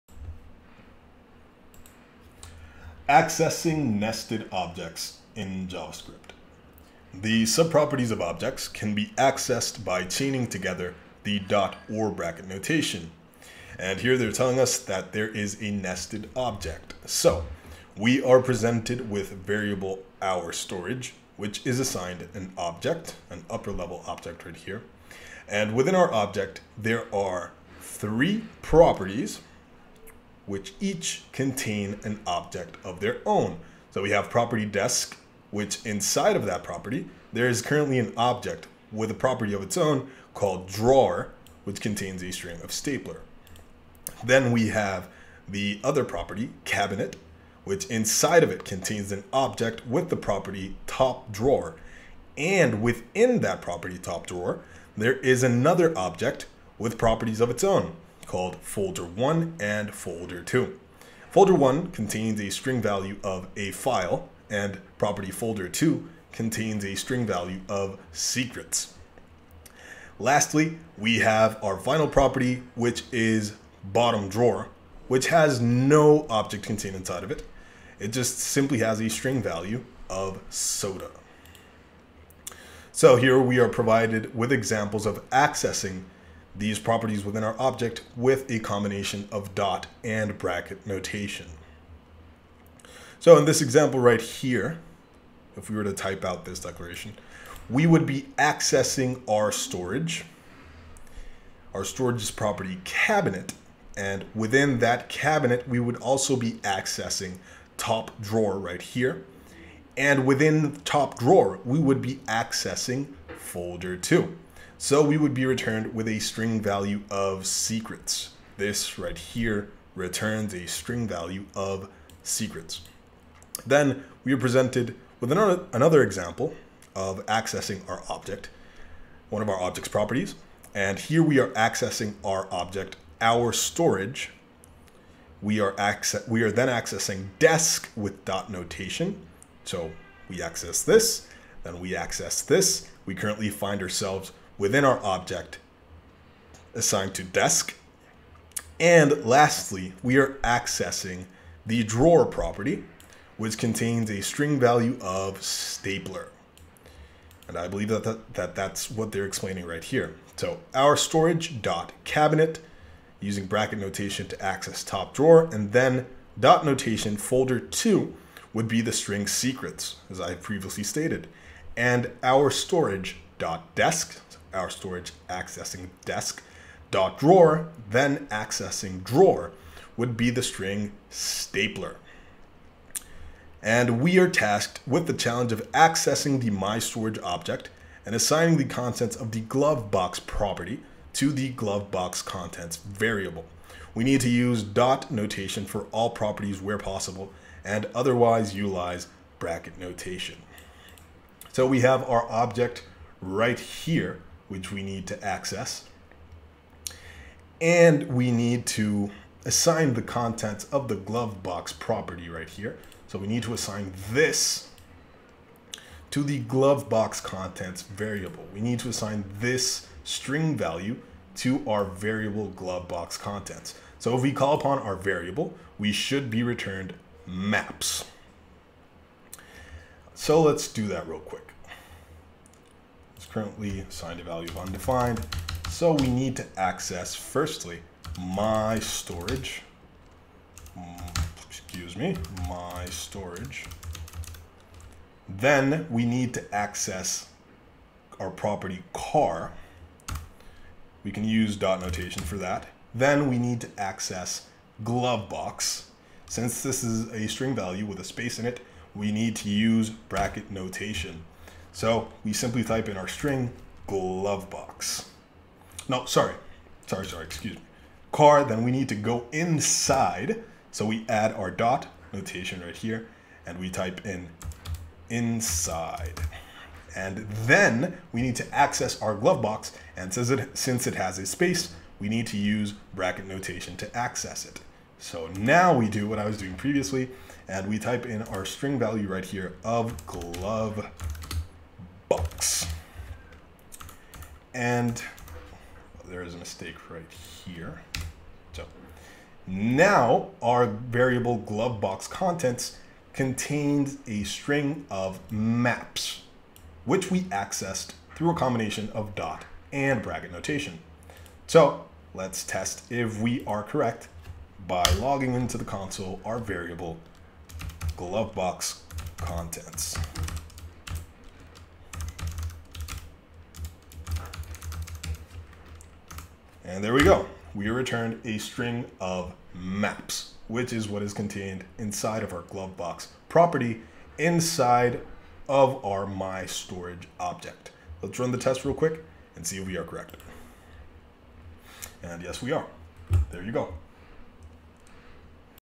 Accessing Nested Objects in JavaScript
By watching this video, you will learn how to access nested objects in JavaScript.